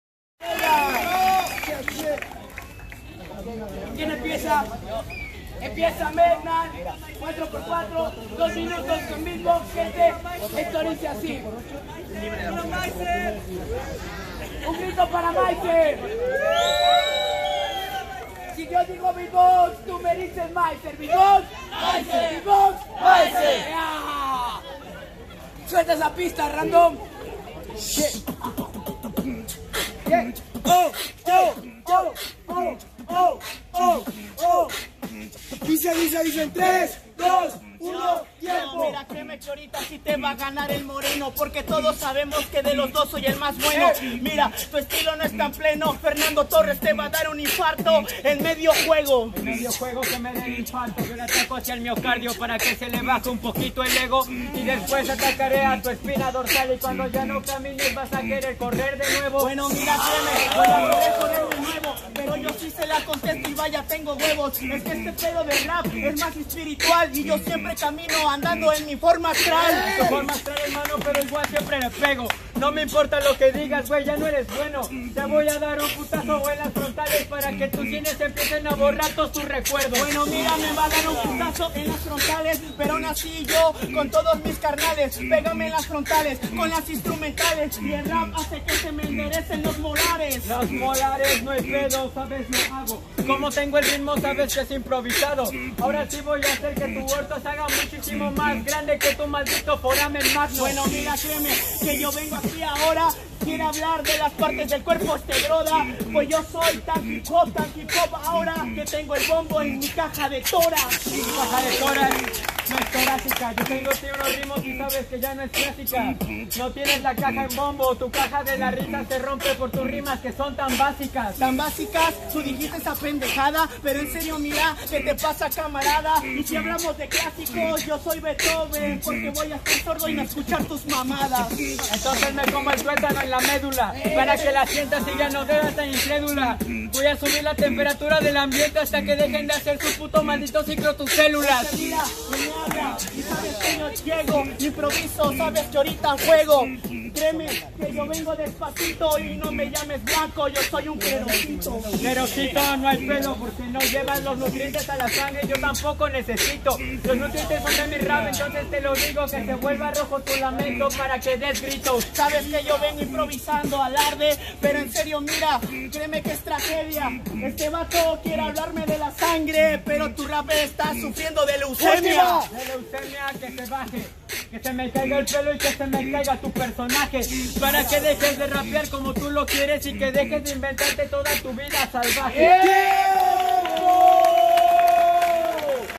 SEMIFINALES / SILVER VS METNAL
ÚLTIMA FECHA
TEMPORADA 0

SEMIFINALES
SILVER VS METNAL

COMPETENCIA PROFESIONAL DE BATALLAS DE RAP UNDERGROUND TIPO FREESTYLE EN XALAPA VERACRUZ MÉXICO.

TODOS LOS DERECHOS DE ESTE MATERIAL QUEDAN COMPLETAMENTE RESERVADOS PARA BRUX Y LOS ARTISTAS QUE APARECEN EN EL MISMO.